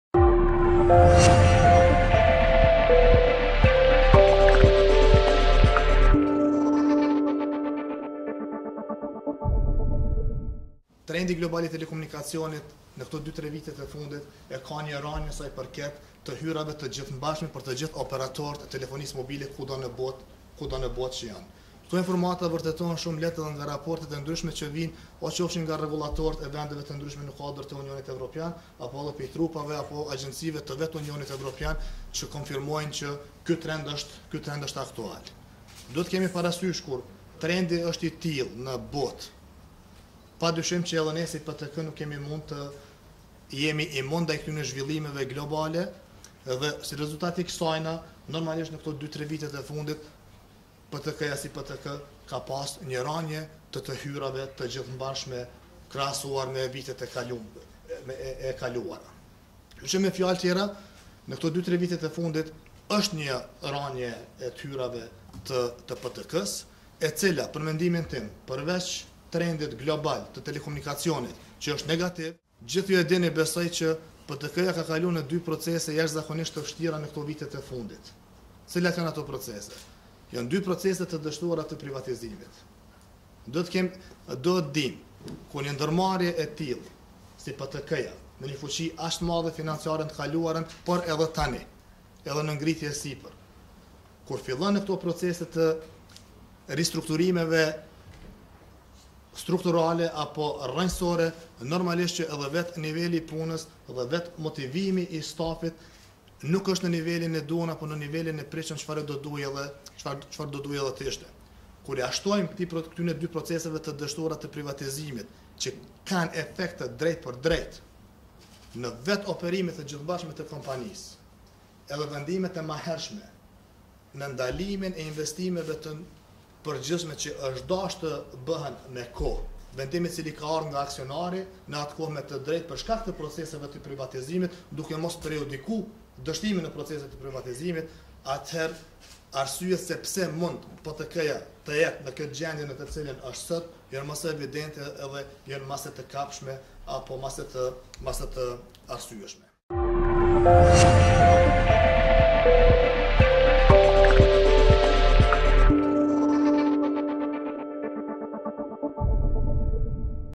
Mustafa: Vizioni im është rikthimi i PTK-së në trend pozitiv
Drejtori menaxhues i Postës dhe Telekomit të Kosovës (PTK), Agron Mustafa, ka thënë se vizioni i tij, është që të ndalë rënien e mëtutjeshme të kompanisë dhe të rikuperojë dëmet nga dy procese të dështuara të privatizimit.